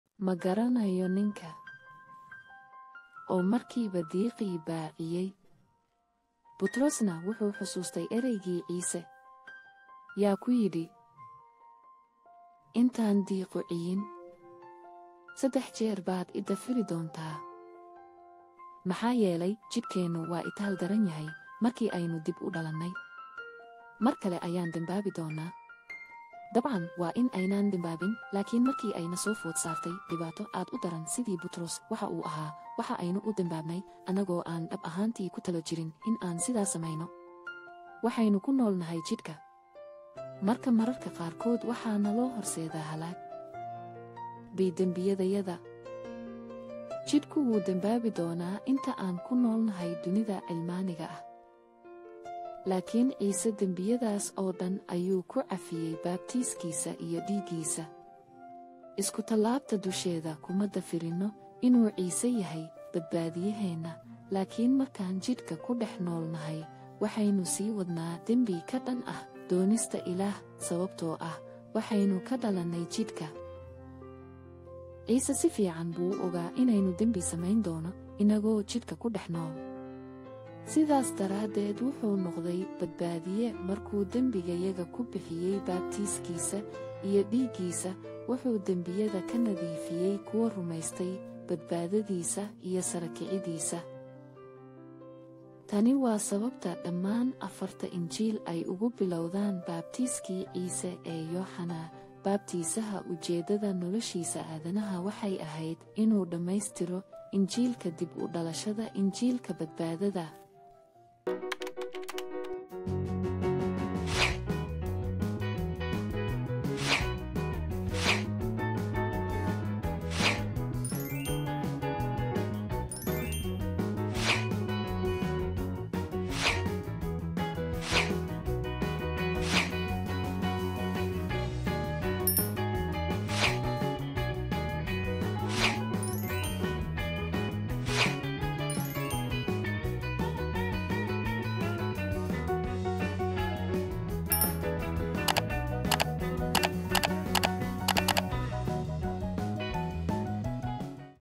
[Barashada Baybalka] Muxuu Butros u diiday Ciise?
We are releasing Paul C. Jong's Christian Book Series absolutely for FREE to every visitor.

Ma garanayo ninka oo markiiba diiqii baa ciyey Butrosna wuxuu xusuustay ereygii Ciise yaa ku yidhi intaan diiqu ciyin saddex jeer baad i dafiri doontaa maxaa yeelay, jidhkeennu waa itaal daranyahay marki aynu dib u dhalannay mar kale ayaan dembaabi doonnaa Dabcan waa in aynaan dembaabin, laakiin markii ay na soo food saartay dhibaato aad u daran sidii Butros waxa uu ahaa waxa aynu u dembaabnay annagoo aan dhab ahaantii ku talo jirin in aan sidaas samayno.
waxaynu ku nool nahay jidhka marka mararka qaarkood waxaa naloo horseedaa halaag by dembiyadayada
Jidhku wuu dembaabi doonaa inta aan ku noolnahay dunida cilmaaniga ah laakiin Ciise dembiyadaas oo dhan ayuu ku cafiyey baabtiiskiisa iyo dhiiggiisa Iskutallaabta dusheeda kuma dafirinno inuu Ciise yahay badbaadiyeheenna, laakiin markaan jidhka ku dhex nool nahay waxaynu sii wadnaa dembi ka dhan ah doonista Ilaah, sababtoo ah waxaynu ka dhalannay jidhka. Ciise si fiican buu u ogaa inaynu dembi samayn doono innagoo jidhka ku dhex nool sidaas daraaddeed wuxuu noqday badbaadiye, markuu dembigayaga ku bixiyey baabtiiskiisa iyo dhiiggiisa, wuxuu dembiyada ka nadiifiyey kuwa rumaystay badbaadadiisa iyo sarakicidiisa.
Tani waa sababta dhammaan afarta injiil ay ugu bilowdaan baabtiiskii Ciise ee Yooxanaa Baabtiisaha ujeeddada noloshiisa aadanaha waxay ahayd inuu dhammaystiro injiilka dib u dhalashada injiilka badbaadada.